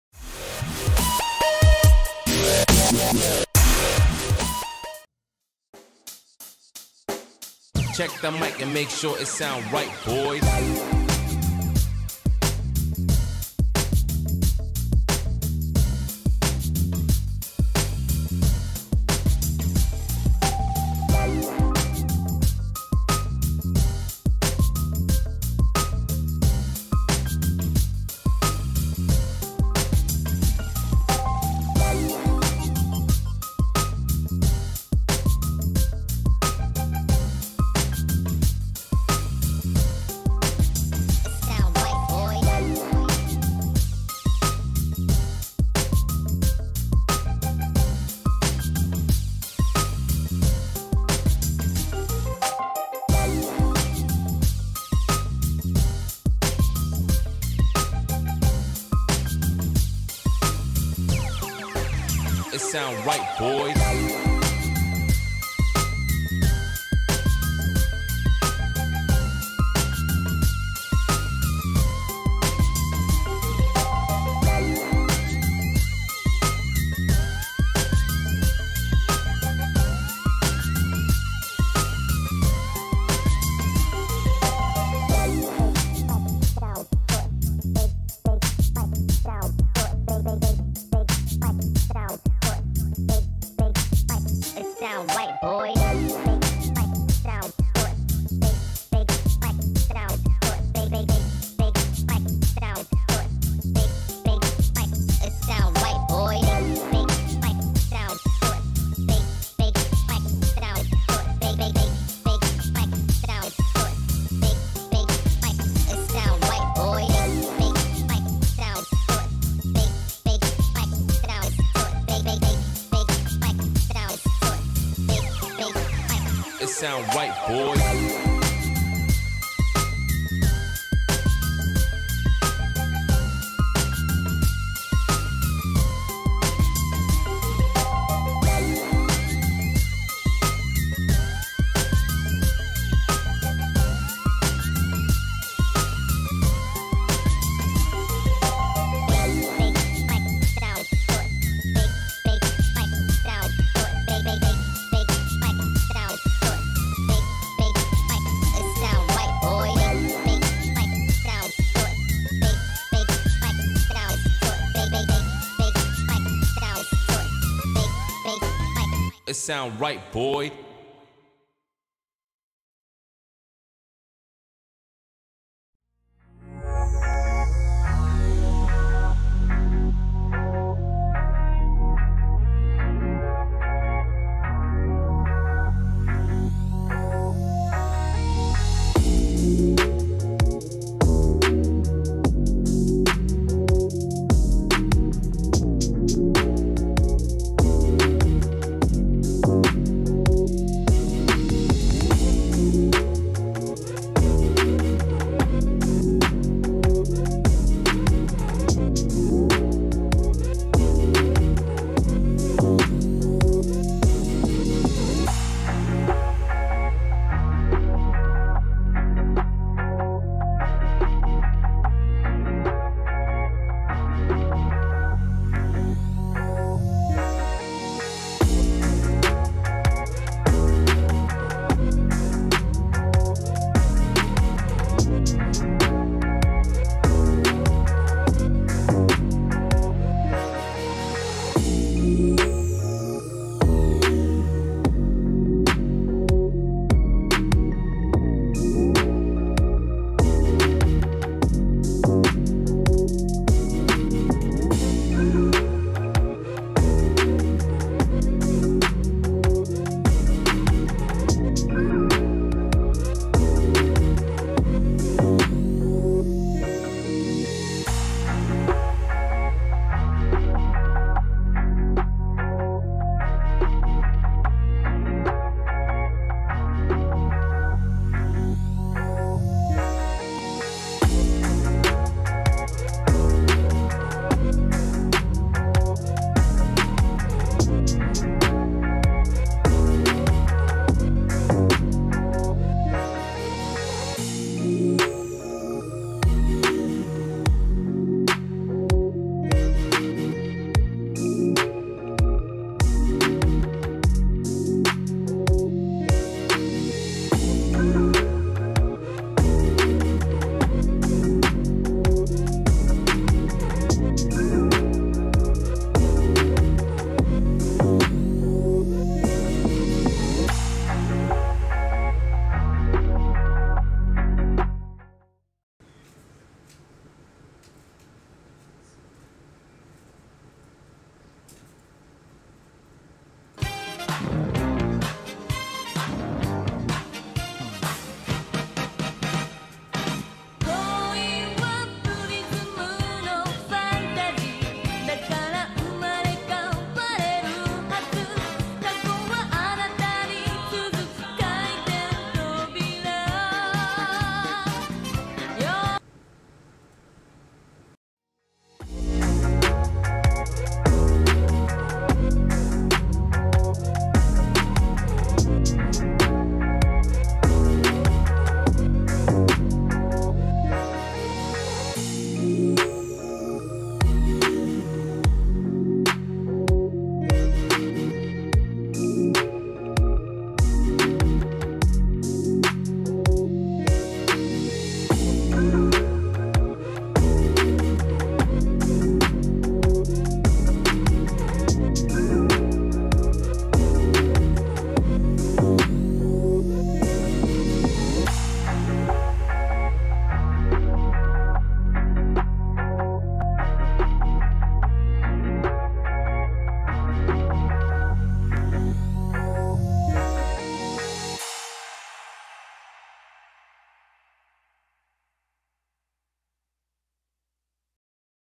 Avexir Core 2 Memory - Armoury Crate / Aura Sync RGB Setup - 2021
Update - might not work on updated versions of armoury crate

Avexir Core 2 Memory RGB syncing process for Aura Sync.
C:\ProgramData\Package Cache\{Insert code here}

0:00  Intro
0:45  How to setup Avexir for Armoury Crate RGB control
2:25  Static Preset
2:55  Breathing Preset
3:18  Strobing
3:40  Color Cycle
4:05  Rainbow
4:45  Starry Night
5:25  Music
6:00  Smart
6:20  Adaptive
6:50  Dark